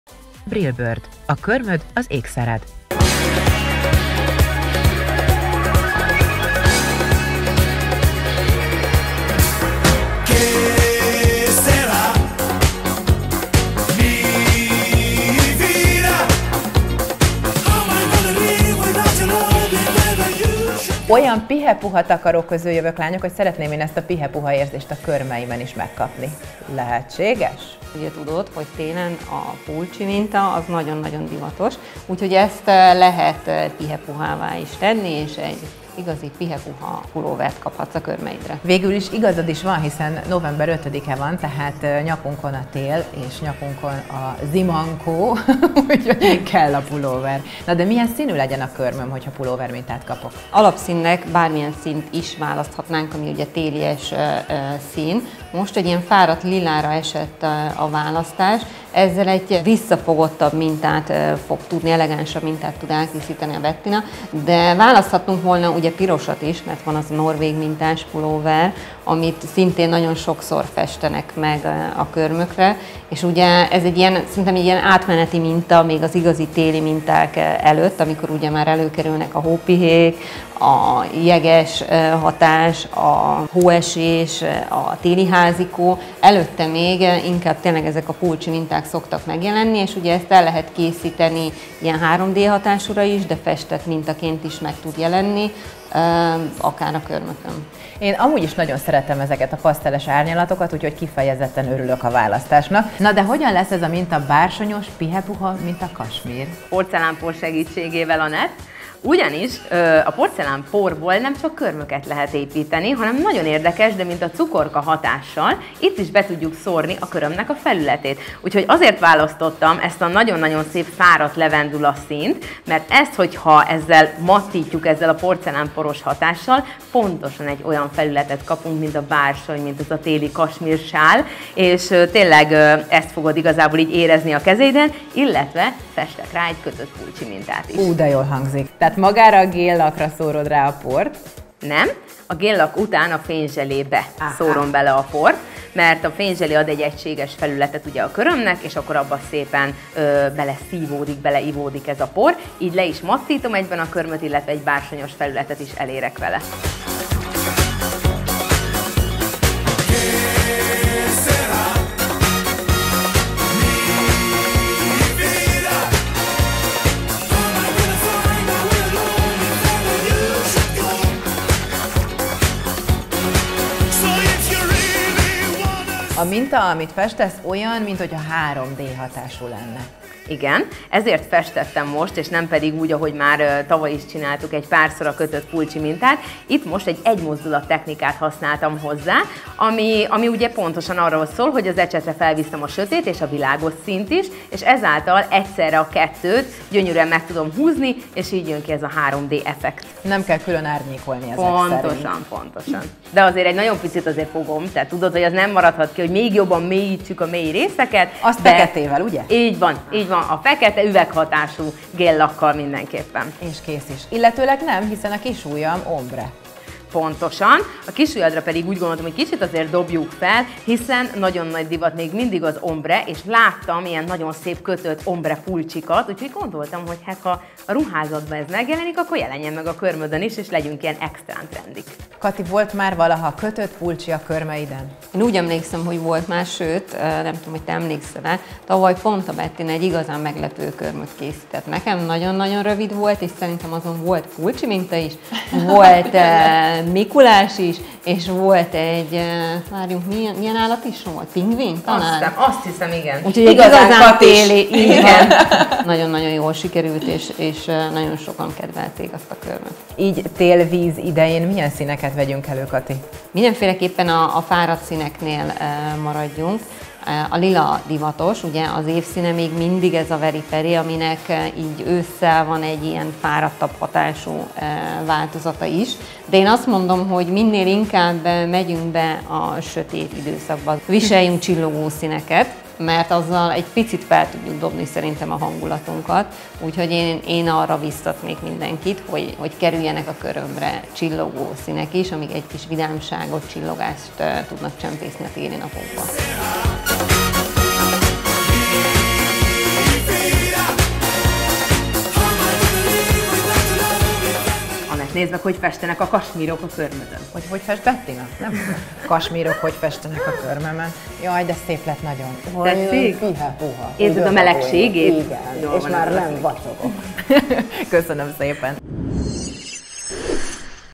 Trendmánia - 3D pulcsi minta - eltérve a megszokottól
Ha már tél, akkor nem maradhat el a pulcsi minta. Bettina egy ilyen körmöt tervezett Anettnek, de természetesen vitt egy kis csavart bele és nem a megszokott mintát  - a megszokott módon készítette el.